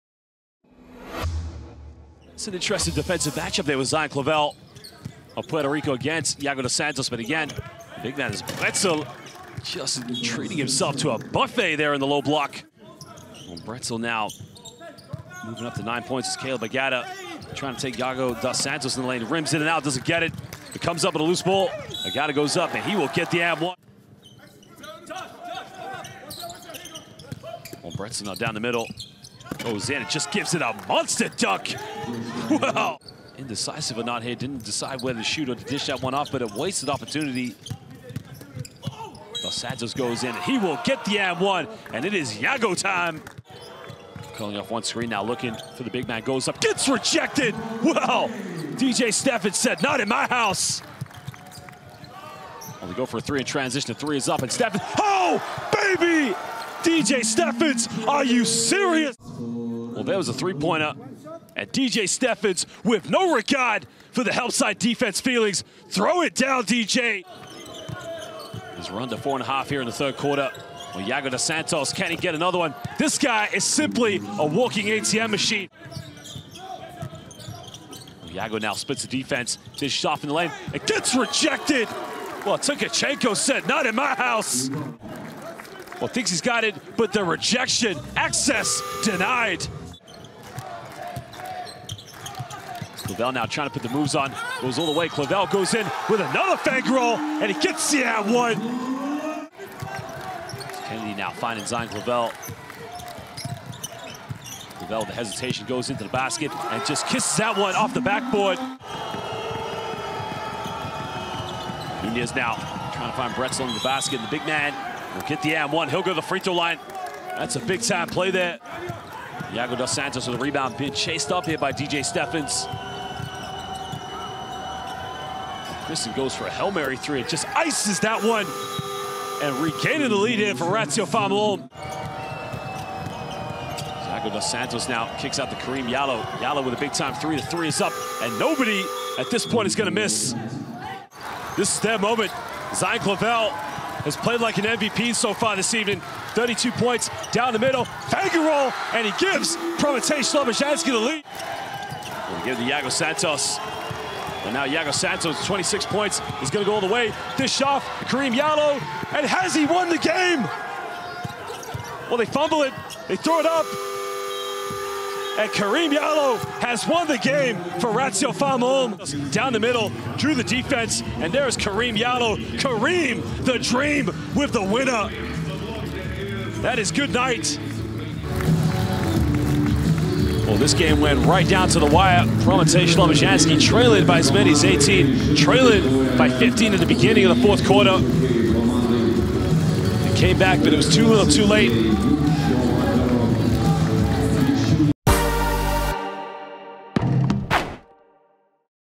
Jallow wins it for Ulm! | Round 6 Highlights |2022-23 7DAYS EuroCup
ratiopharm Ulm has improved its record in the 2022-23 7DAYS EuroCup to 3-3 after beating Prometey Slobozhanske 105-106 at Zalgirio Arena in Kaunas, Lithuania, on Wednesday evening. This defeat means that Prometey now finds itself 3-3.